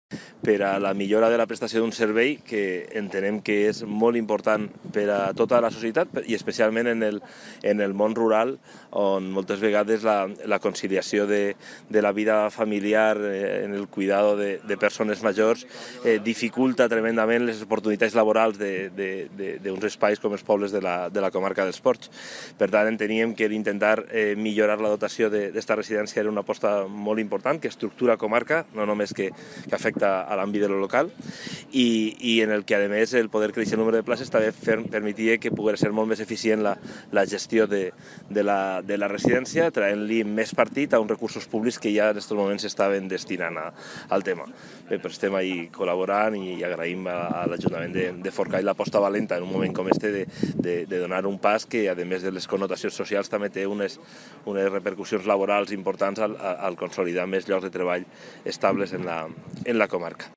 Moliner explica a los mayores usuarios de la Residencia de Forcall las obras de ampliación
El presidente de la Diputación de Castellón, Javier Moliner, ha explicado a los mayores usuarios de la Residencia de Forcall las obras de mejora y ampliación en las que la institución provincial invierte 120.000 euros a través de un convenio de colaboración con el Ayuntamiento del municipio. La aportación provincial, que representa el 70,41% del presupuesto total de la obra, de 170.407 euros, se ejecuta bianualmente en los ejercicios 2014 y 2015. El presidente provincial ha puesto en valor la importancia de invertir en este centro que supone para el pueblo y la comarca el mantenimiento de un total de 27 puestos de trabajo.